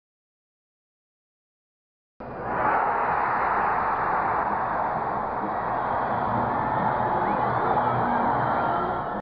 Taylor Twellman volley
Twellman left footed volley goal in 2002.